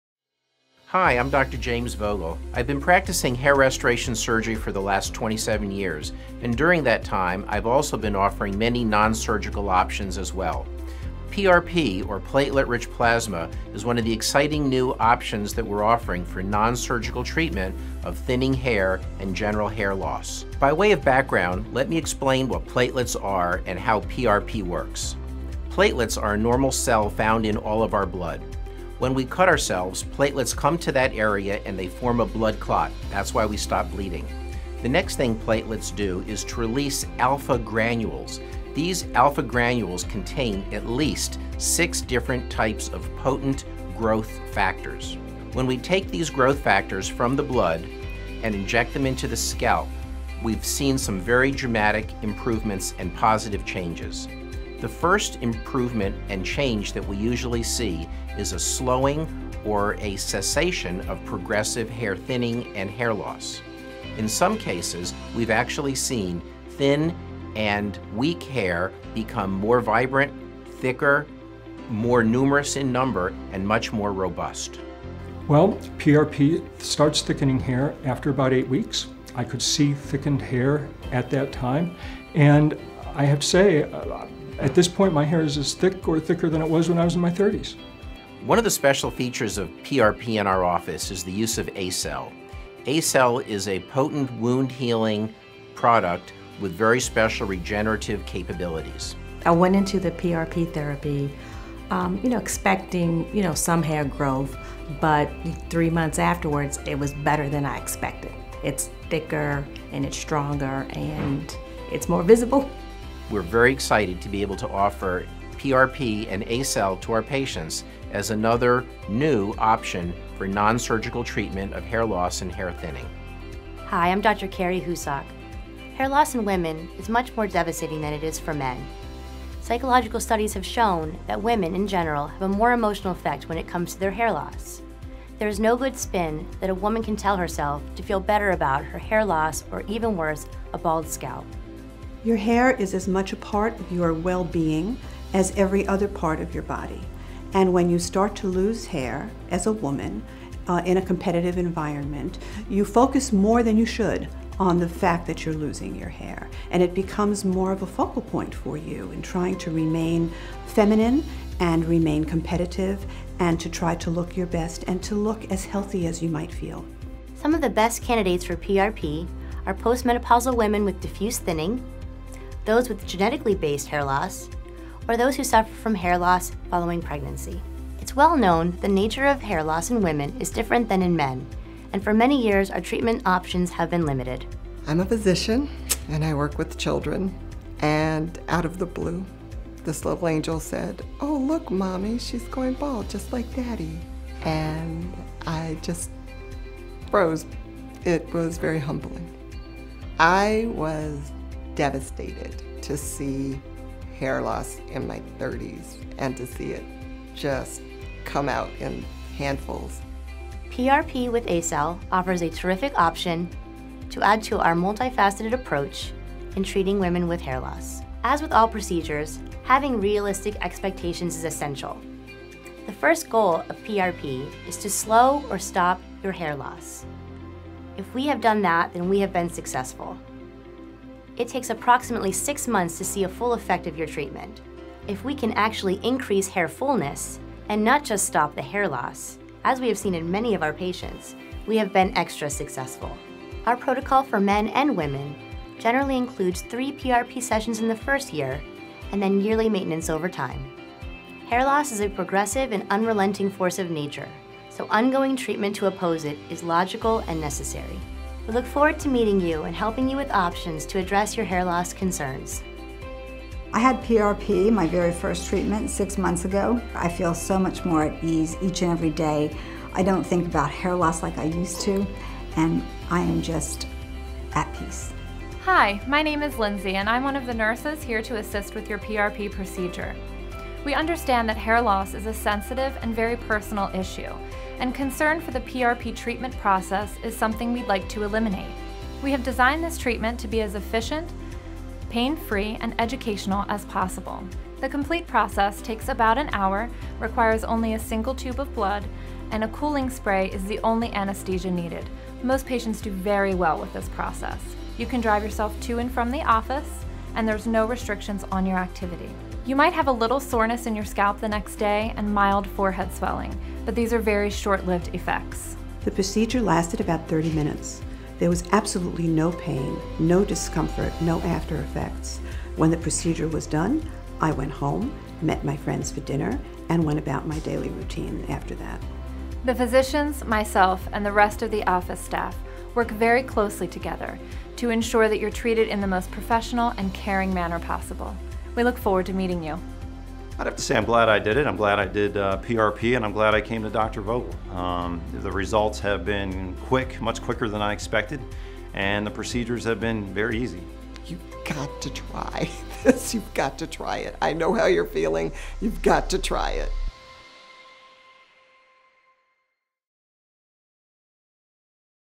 The Platelet Rich Plasma (PRP) & Cell® Injection Therapy Procedure - Dr. James E. Vogel, MD, FACS
Doctors James Vogel and Carrie Houssock discuss hair restoration treatment using Platelet Rich Plasma (PRP) and ACell® injection therapy. This injectable therapy for hair loss is FDA-approved.